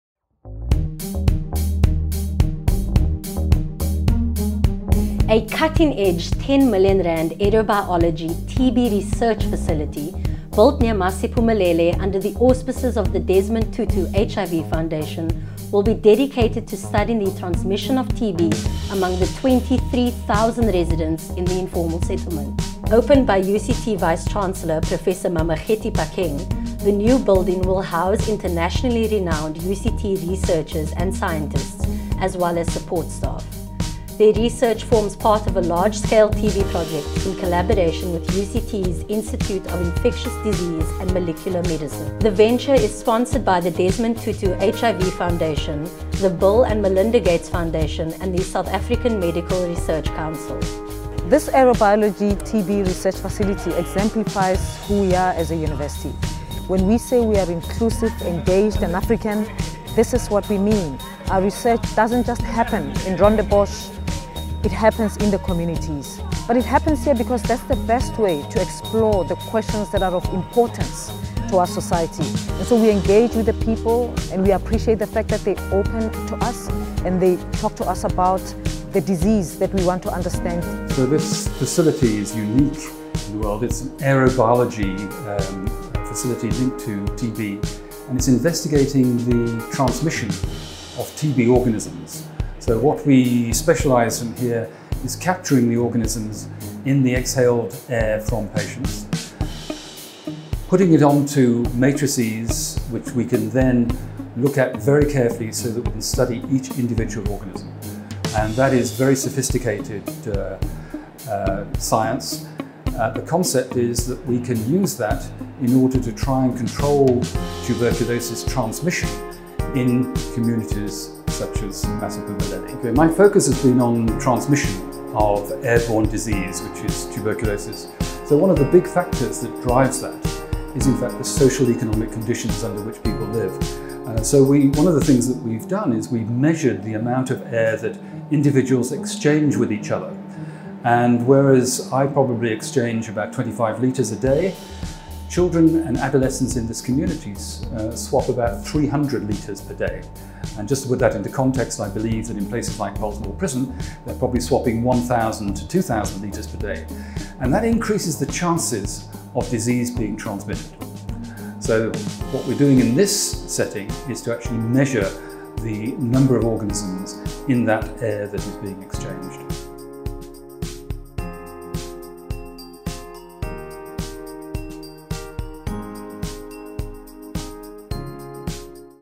UCT launches Aerobiology TB Research Facility in Masiphumelele
The cutting-edge Aerobiology TB Research Facility, built near Masiphumelele and dedicated to studying the transmission of tuberculosis, was opened by UCT Vice-Chancellor Professor Mamokgethi Phakeng on 20 February 2019.